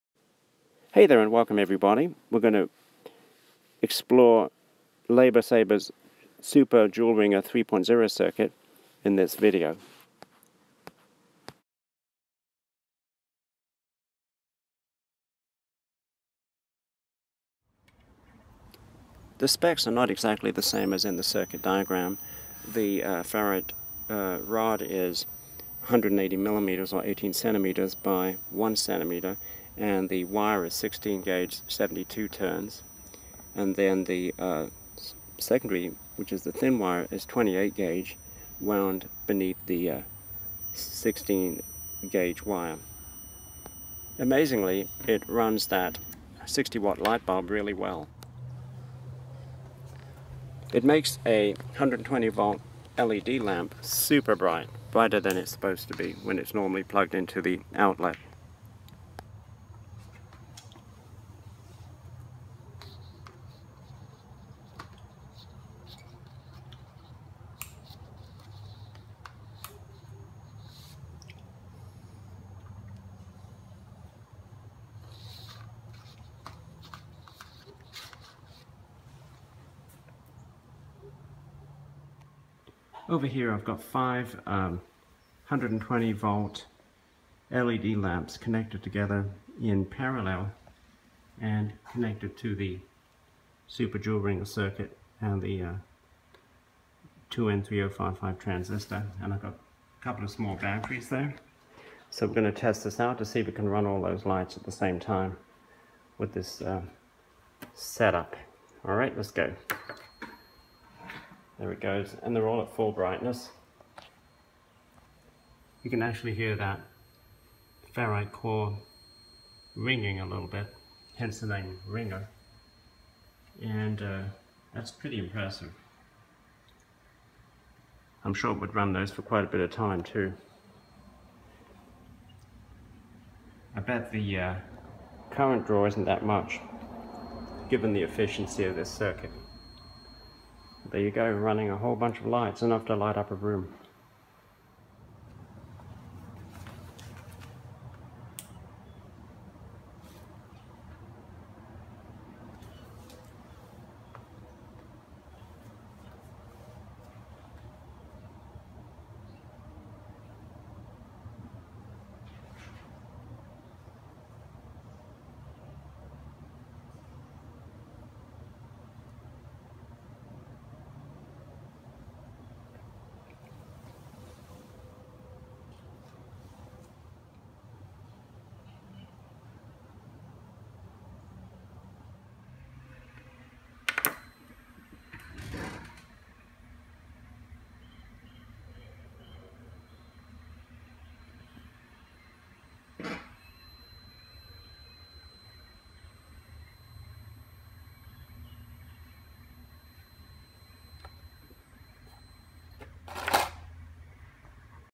Evaluation of Super Joule Ringer - Amazing results!
Super Joule ringer setup using 3 components battery, ferrite rod transformer, and transistor. Can light 120V  incandescent light bulbs, and multiple parallel connected 120V  LED bulbs. Even wirelessly lights LEDs. The specs for the ferrite core transformer were different to that of the original Super Joule Ringer 3.0 circuit of LaserSaber, but  it worked suprisingly well.